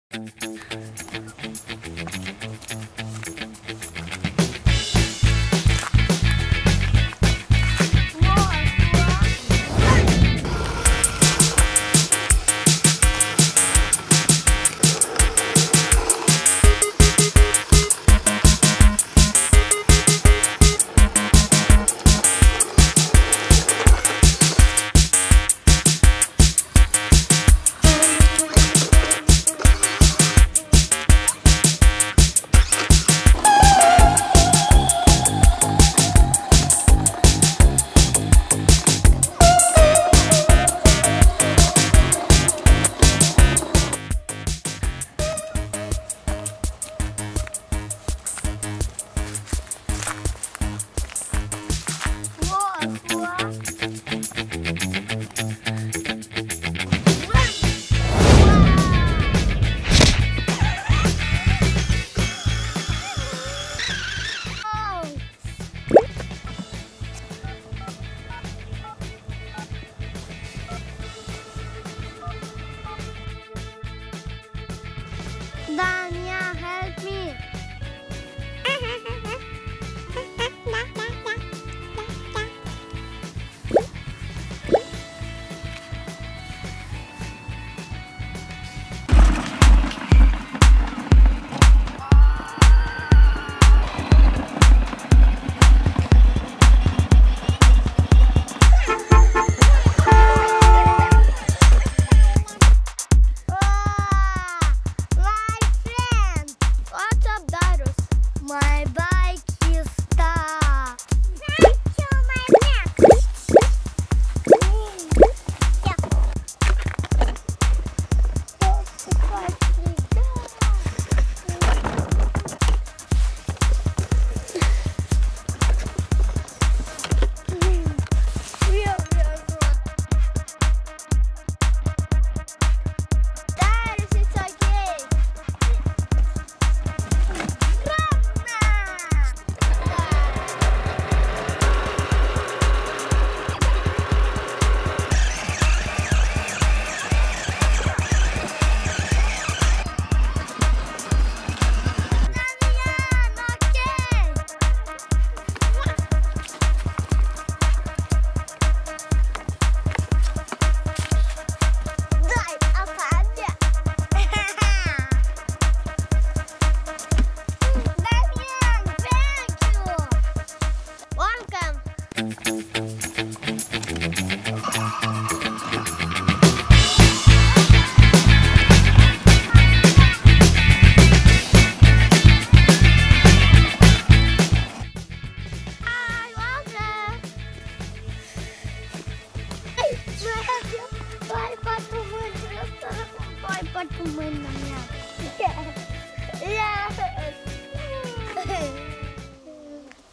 Funny Kids Ride on Power Wheels Cross Bikes Helping each other / Children's Bike / Biker Toys
Funny story about brother's adventure :)

✰ Enjoy our instagram pages also, so we can get to know each other better :)